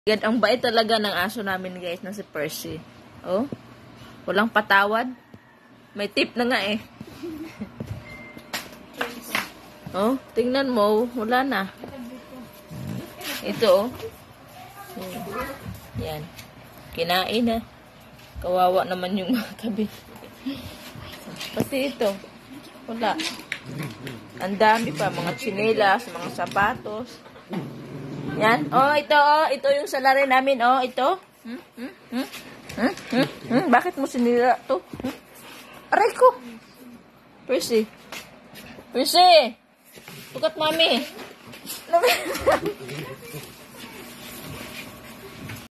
ang salarin sa sumira nang gamet namin aso na subrang kulit
An salarin na sumira nang mga gamet namin ang aso na makulit